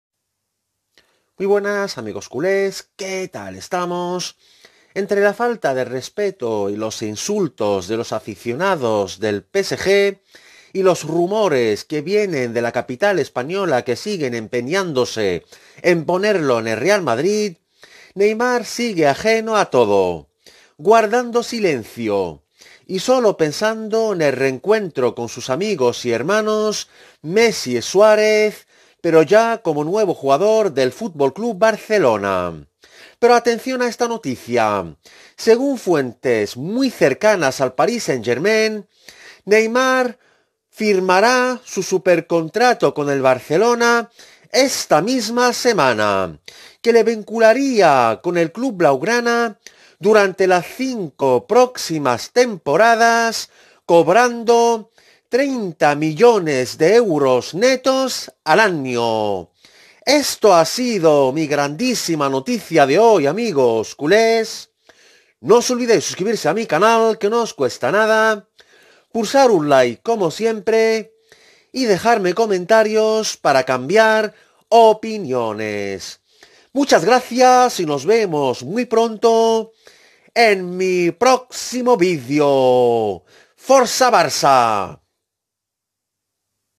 OFICIAL: ¡¡EL NUEVO SUPER CONTRATO DE NEYMAR CON EL BARCELONA Y SU NUEVA SUPER FICHA!!
La directiva del Barcelona encabezada por Éric Abidal ya estan en París. Para acordar el super contrato de Neymar y la cifra astronómica que va a cobrar